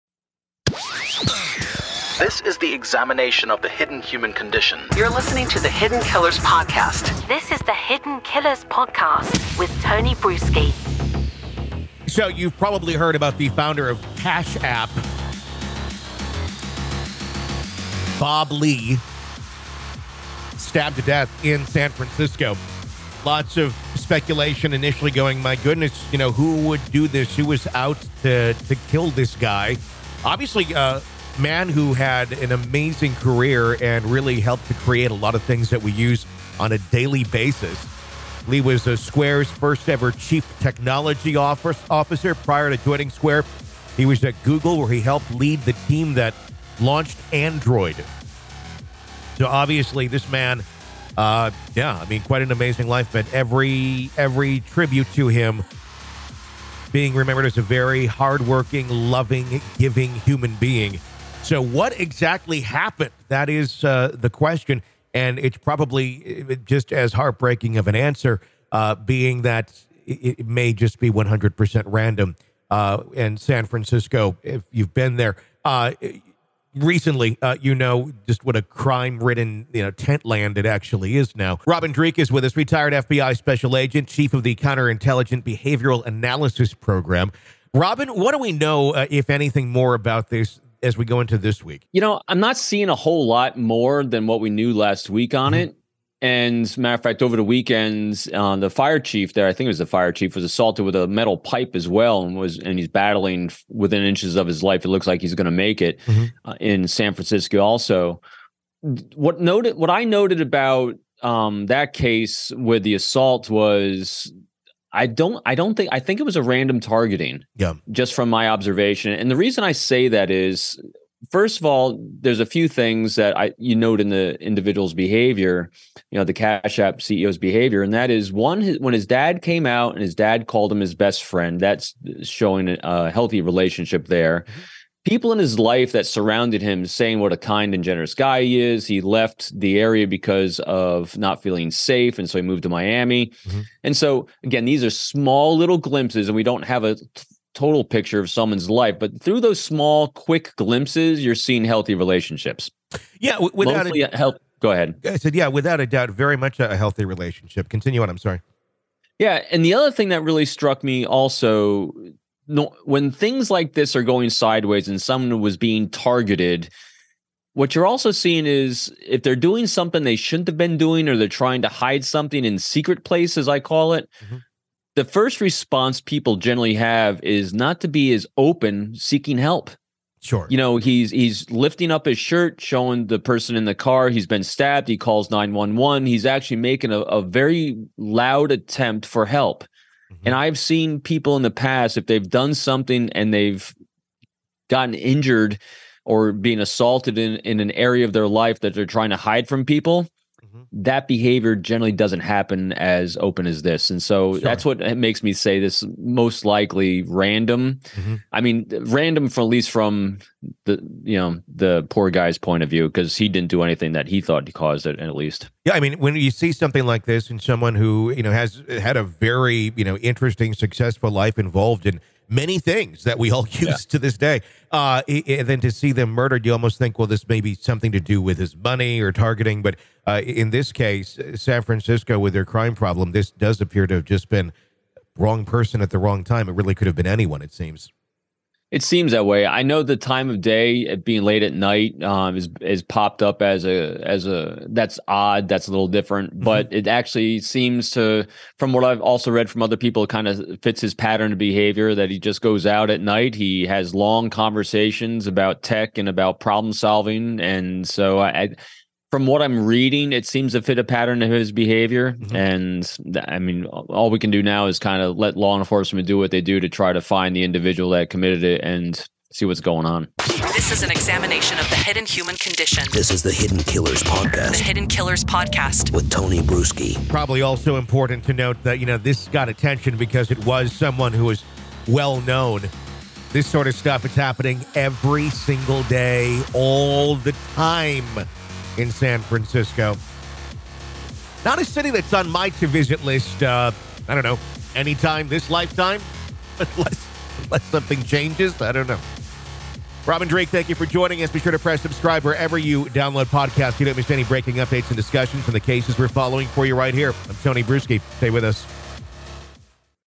Unanswered Questions: What Led to the Tragic Stabbing of MobileCoin's Bob Lee?
Tech innovator Bob Lee, who played a crucial role in launching Cash App and later joined MobileCoin as its chief product officer, was tragically killed in a stabbing attack in San Francisco. The incident has left the tech community and the public searching for answers as to why it happened. As local authorities continue their investigation into Lee's death, the city grapples with a recent uptick in crime and mourns the loss of a bright and compassionate figure.

Chad & Lori Daybell -

The Murder of Ana Walshe -

Alex Murdaugh -

The Idaho Murders, The Case Against Bryan Kohberger -

Lucy Letby -